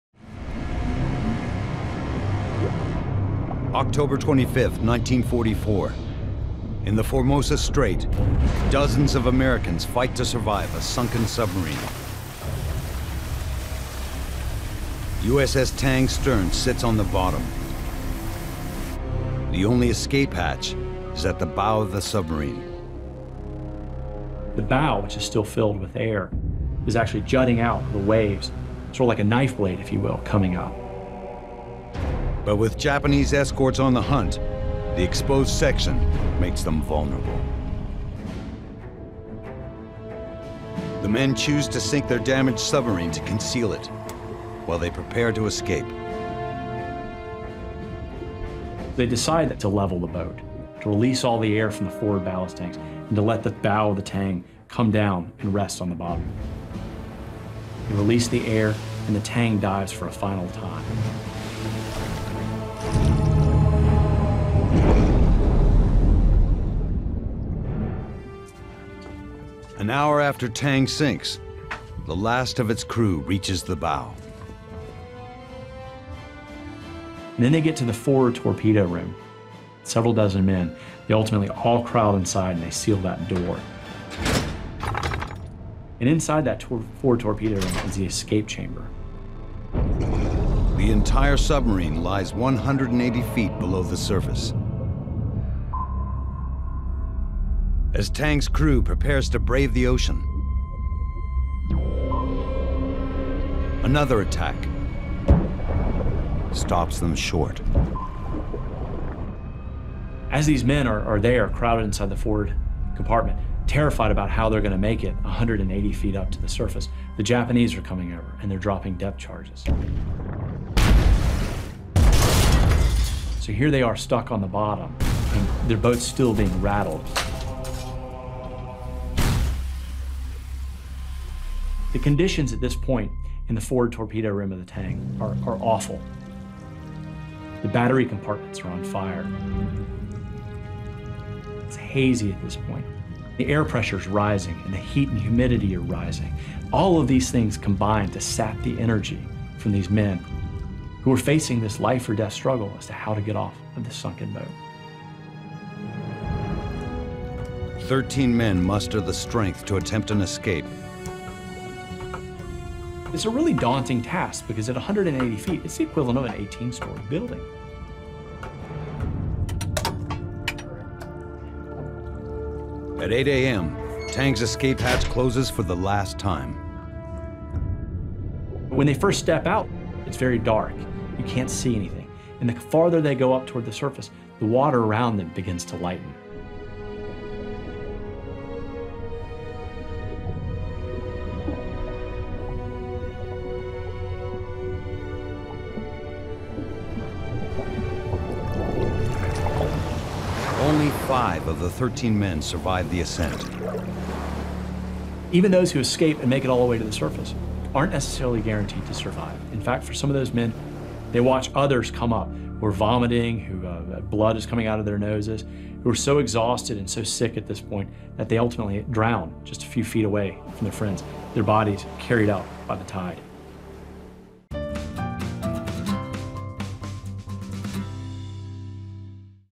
How to Escape a Sunken Submarine Alive
The USS Tang has been sunk by a Japanese gunboat, but a handful of its crew remain alive inside. Their only chance of survival is to open the hatch and swim 180 feet to the surface.